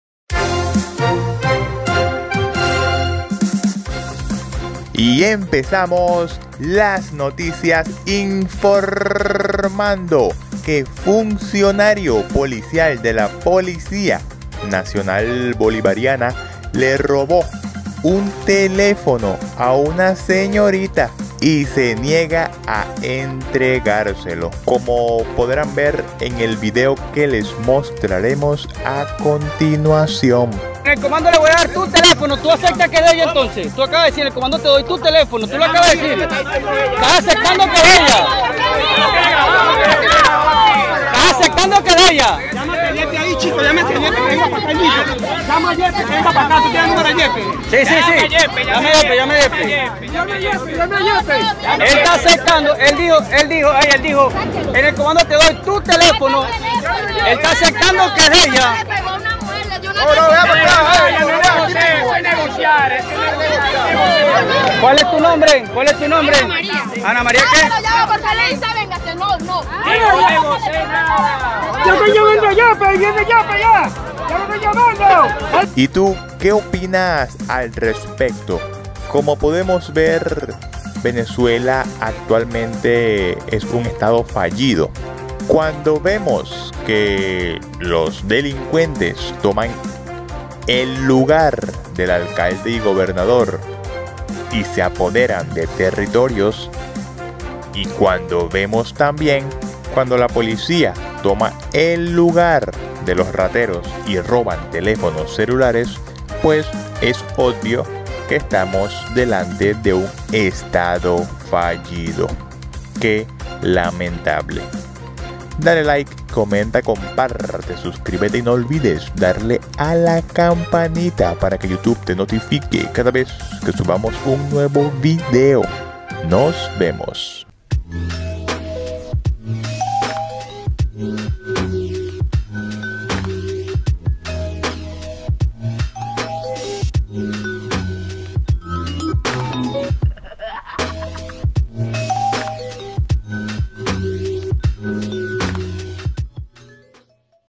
INSOLITO: MIRA COMO UN FUNCIONARIO POLICIAL LE QUITA EL TELÉFONO A UNA CIUDADANA Y NO SE LO REGRESA
Mira este insolito video de in PNB (policia nacional bolivariana) le quita el teléfono a una mujer y no se lo devuelva.